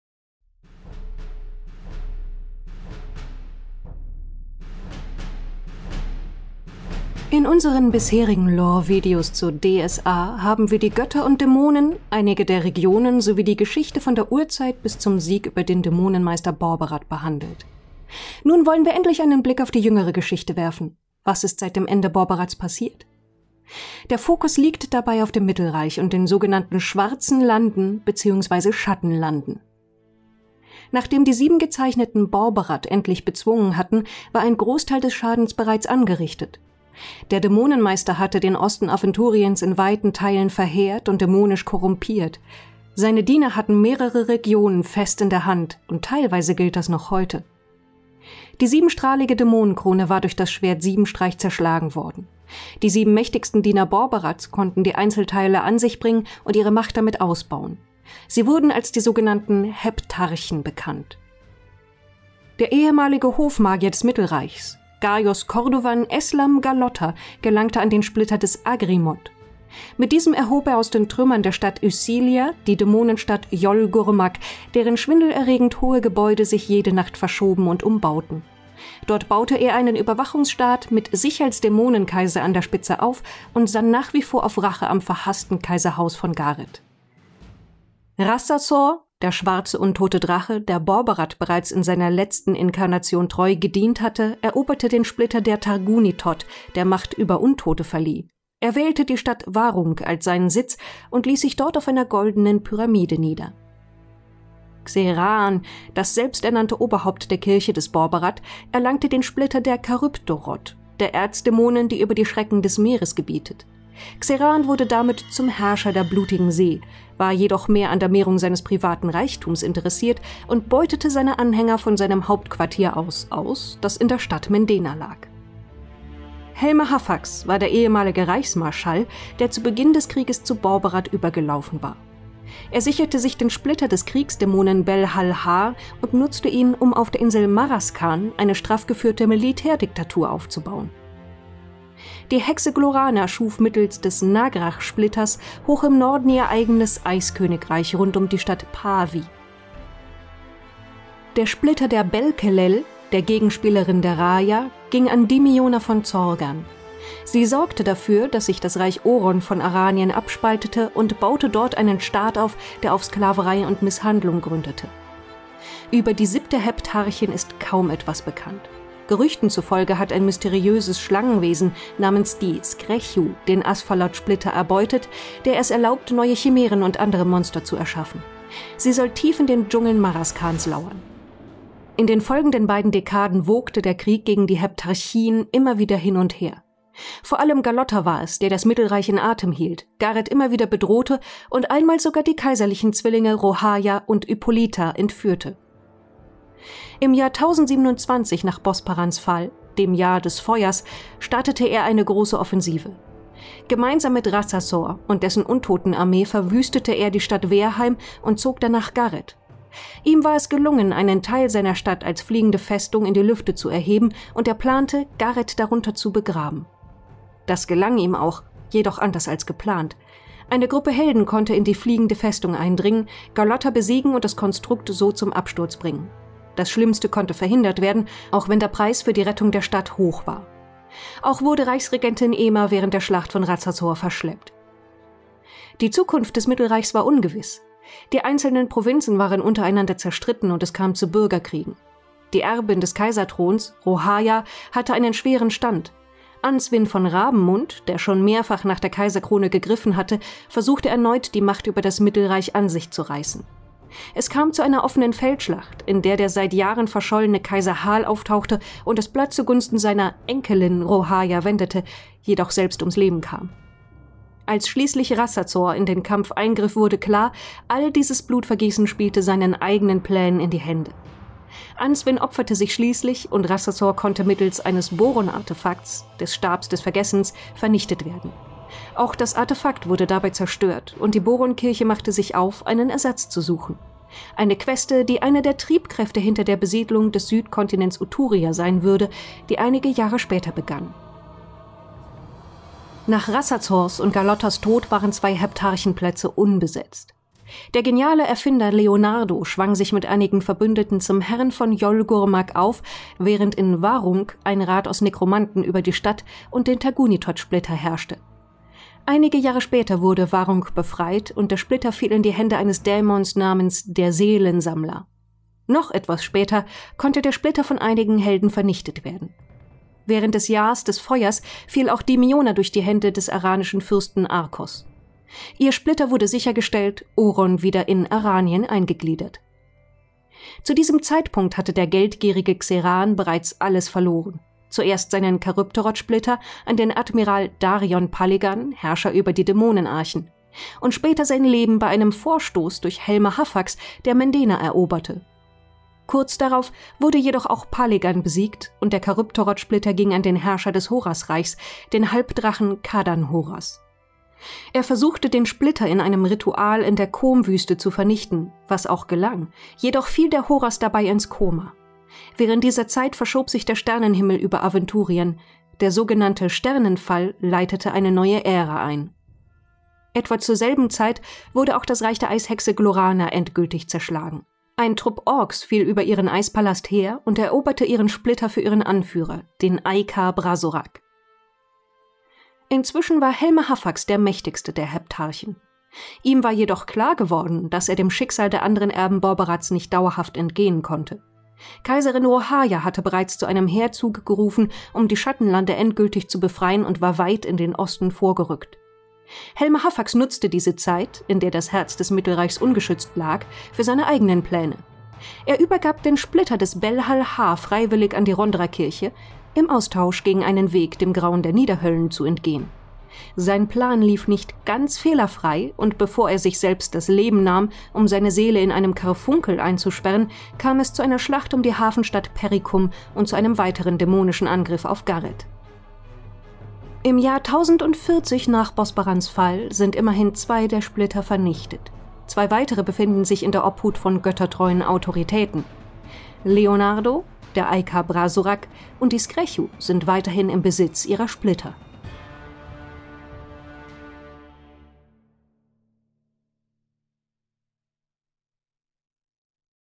Ein Blick ins Schwarze Auge: Die Heptarchen - Borbarads Erben (Lore-Video für Einsteiger)
In unserer lange laufenden Reihe von DSA-Hintergrundvideos und Tutorials geht es heute endlich um die Zeit nach Borbarad. Das Video enthält natürlich SPOILER!
Wenn ihr uns unterstützen und hinter die Kulissen schauen wollt, geht das